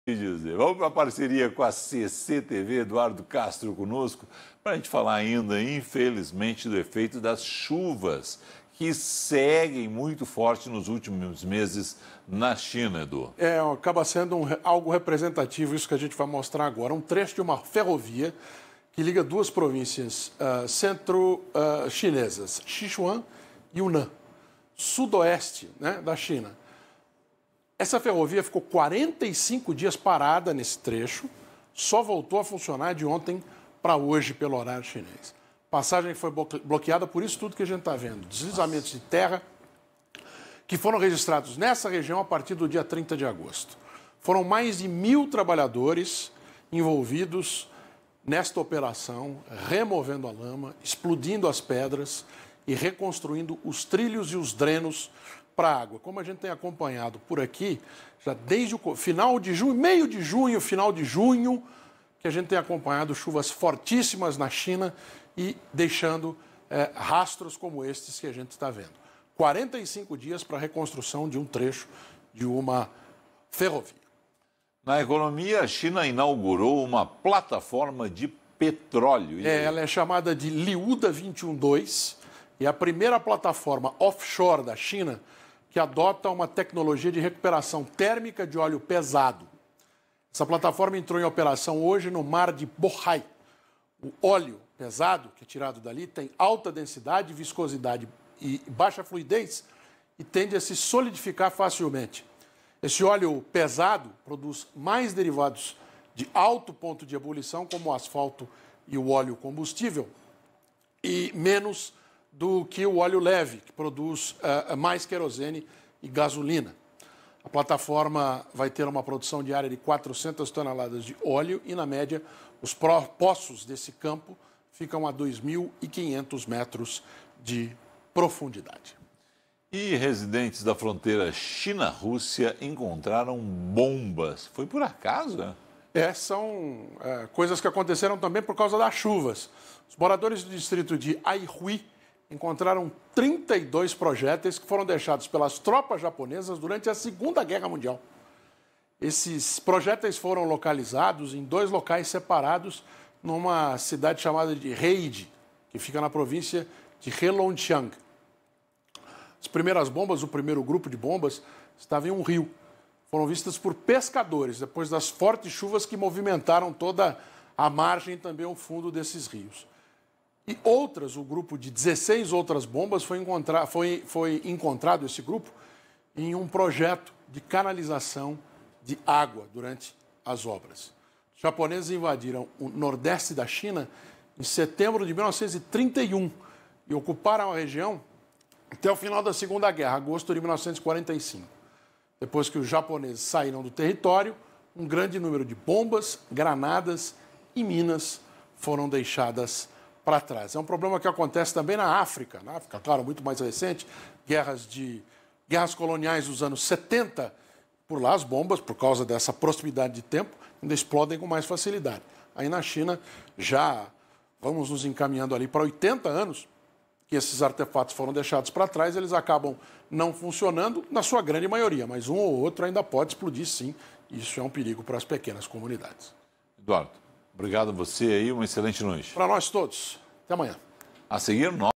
Eduardo Castro mostra os destaques da China
O jornalista Eduardo Castro mostra os destaques da China no nosso giro de notícias feito em colaboração com a CCTV, parceira do BandNews.

Baixe o nosso aplicativo para mais notícias!